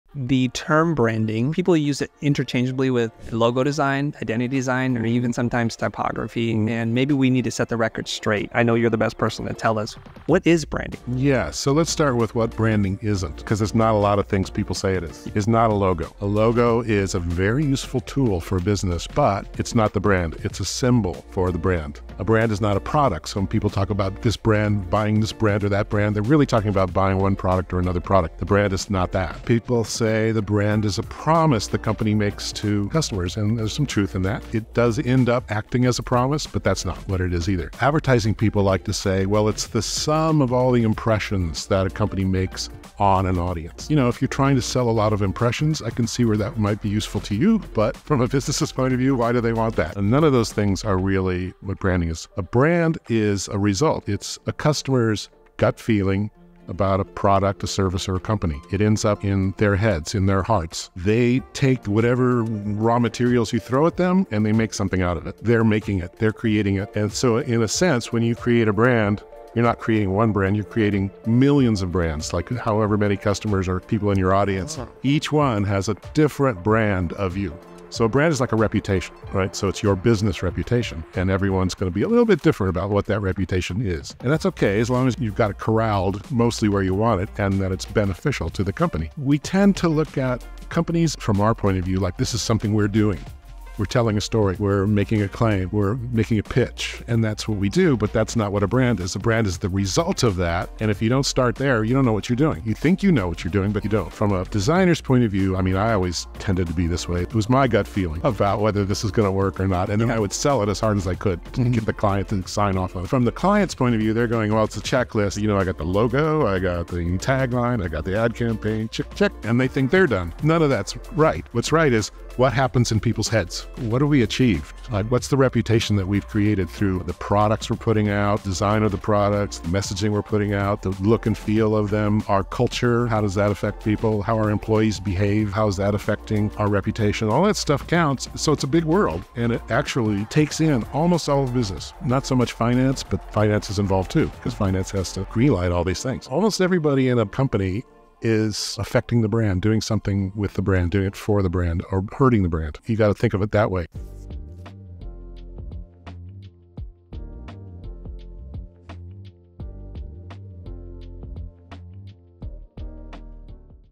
What Is Branding? 3 Minute Crash Course.
What is branding? A brand is not a logo. A brand is not a product. A brand is not a promise. A brand is not the sum of all the impressions it makes on an audience. A brand is a result–it's a person's gut feeling about a product, service or company. It's in their heads and in their hearts. A brand is your reputation.

Marty Neumeier started as a graphic designer and copywriter in the 1970s. In 1984, when the Macintosh launched, he moved to Silicon Valley to help companies like Apple, Netscape, HP, Adobe, and Google build their brands.

"Your brand isn’t what you say it is. It’s what they say it is."

Books by Marty Neumeier
The Brand Gap
The Designful Company
The 46 Rules Of Genius
The Brand Flip

We love getting your letters. Send them here:
556 S. Fair Oaks Ave. #34

Host: Chris Do
Producer: Mark Contreras
Cinematographers/Editors: Stewart Schuster @RodrigoTasca &  @Tascastudios  MOCS Media